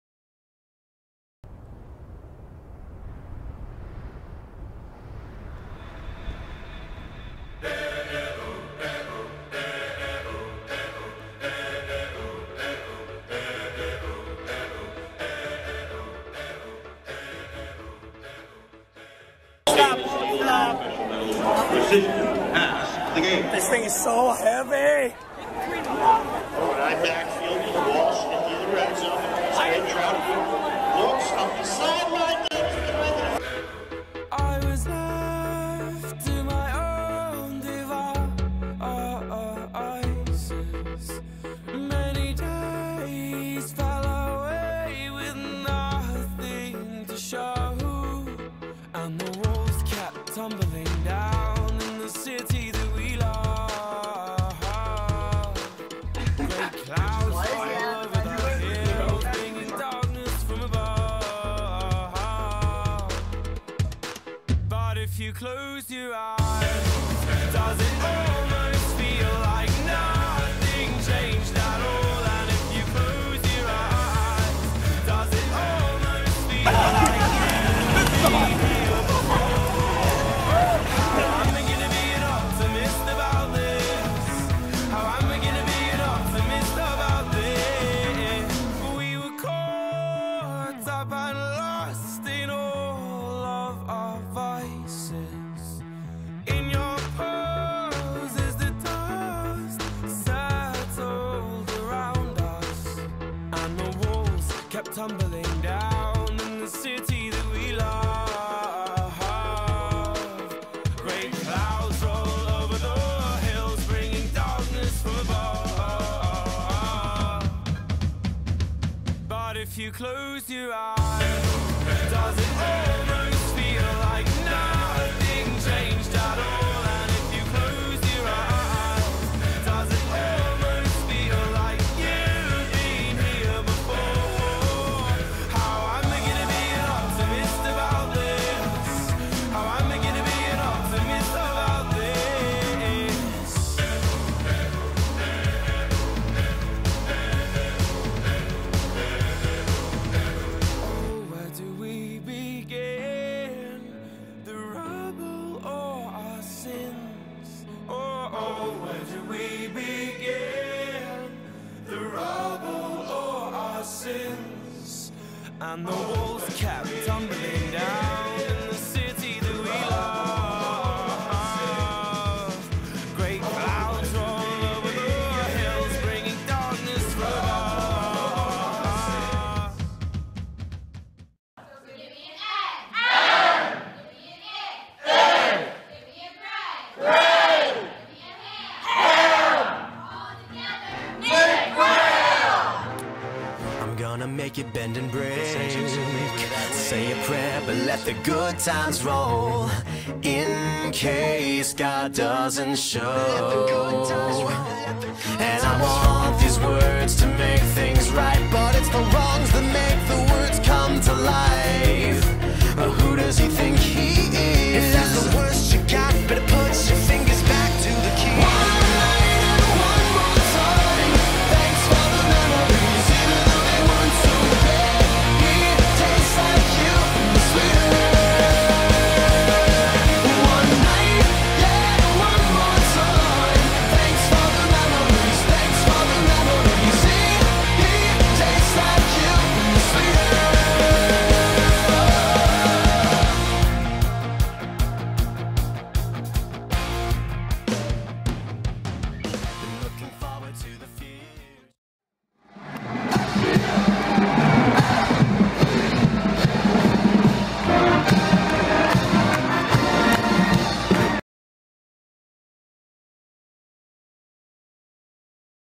Spirit of Stony Brook - Fall 2014
Historian Luis Tobon captured moments of the Spirit of Stony Brook throughout the fall 2014 semester.